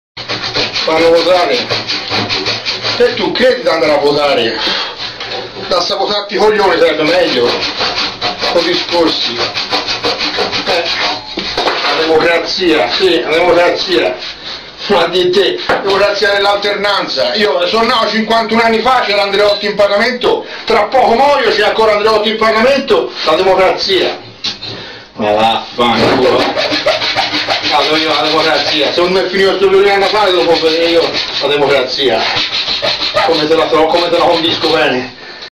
Democrazia
Ho estrapolato da un video del liutaio, Fabio Chiari, un piccolo pensiero che egli esprime in merito ad un discorso con il suo allievo sulla democrazia.